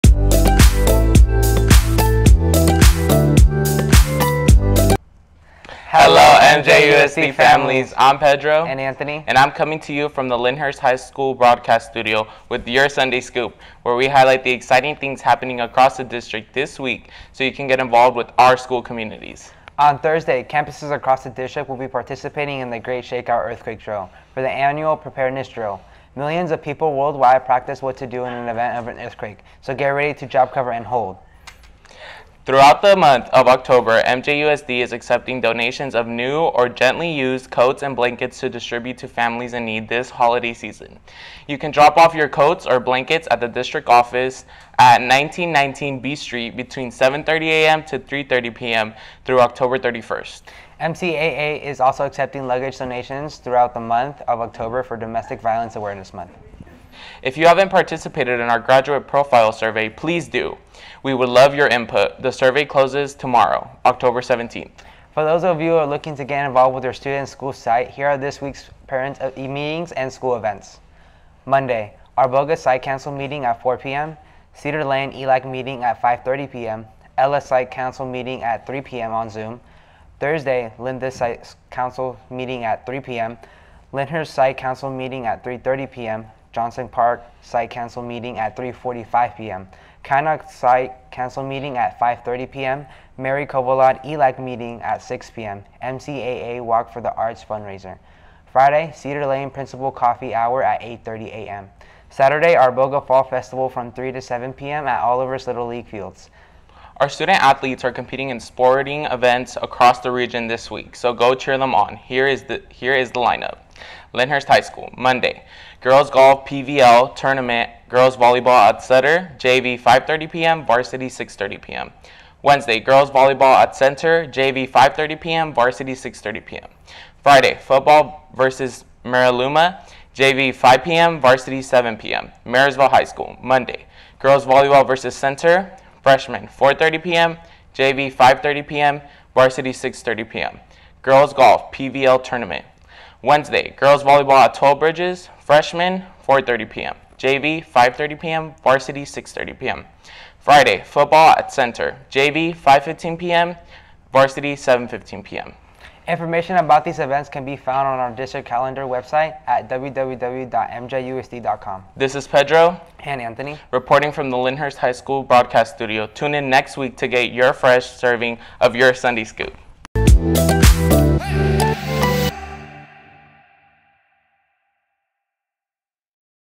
Sunday Scoop 10/16/22 - English
The Marysville Joint Unified School District's Sunday Scoop is a weekly student-produced broadcast that highlights news and previews upcoming events.